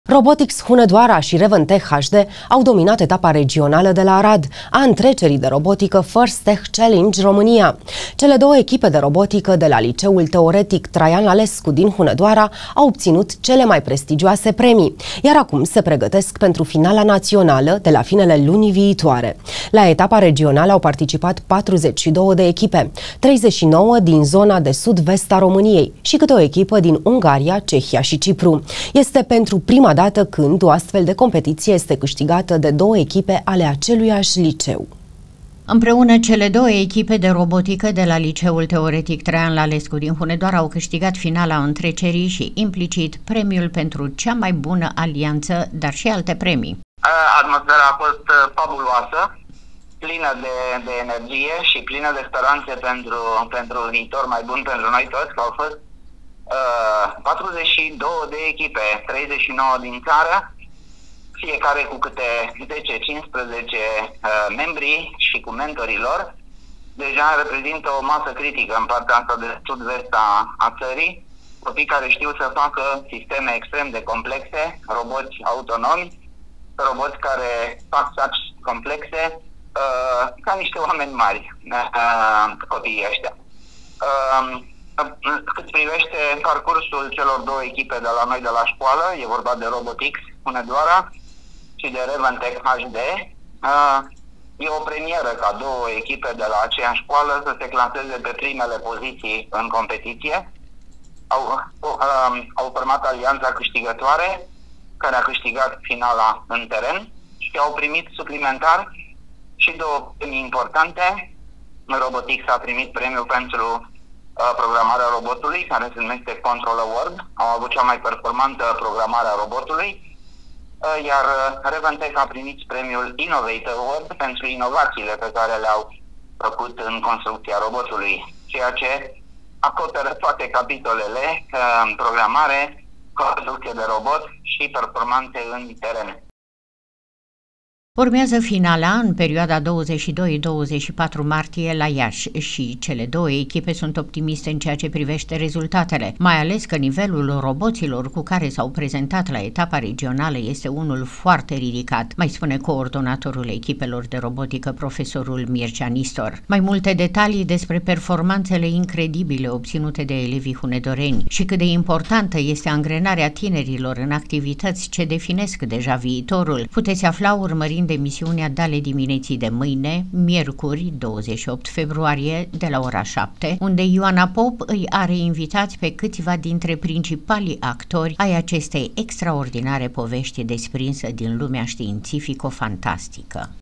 ROBOTX ŞI RAVENTECH HD DE LA „TRAIAN LALESCU” DIN HUNEDOARA
AU DOMINAT ETAPA REGIONALĂ A ÎNTRECERII DE ROBOTICĂ „FIRST TECH CHALLENGE” ROMÂNIA

RobotX Hunedoara  şi RavenTech HD  au dominat etapa regională de la Arad, a întrecerii de robotică „First Tech Challenge” România. Cele două echipe de robotică de la Liceul Teoretic „Traian Lalescu” din Hunedoara au obţinut cele mai prestigioase premii, iar acum se pregătesc pentru finala naţională, de la finele lunii viitoare. La etapa regională au participat 42 de echipe - 39 din zona de Sud-Vest a României şi câte o echipă din Ungaria, Cehia şi Cipru. Este pentru prima dată când, o astfel de competiție,este câștigată de două echipe ale aceluiași liceu.